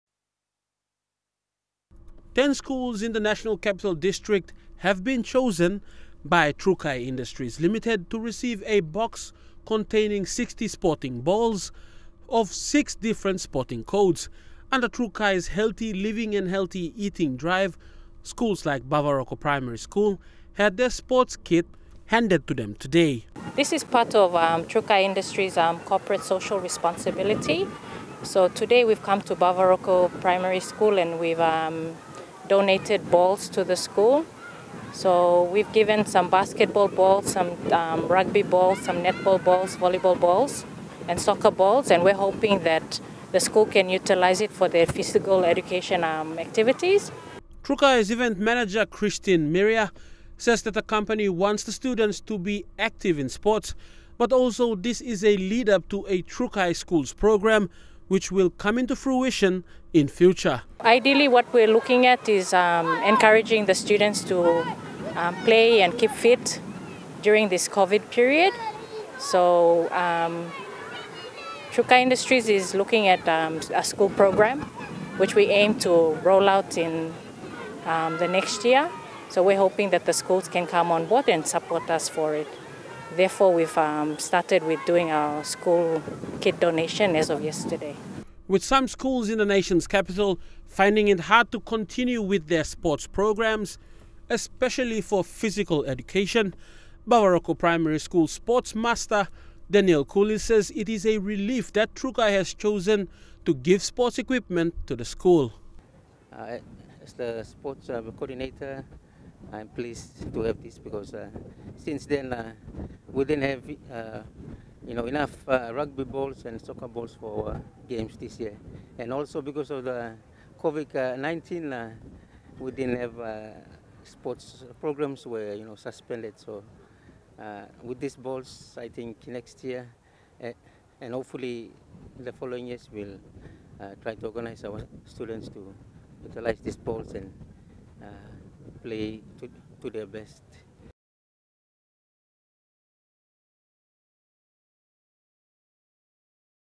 Trukai Sports Kit
Keeping fit and healthy is also part of the school curriculum and Trukai Industries Limited has embarked on a Sports Kit Donation drive to schools within Port Moresby.

This is under Trukai's healthy living and healthy eating platform.
In this program, 10 schools have been identified to receive the donation.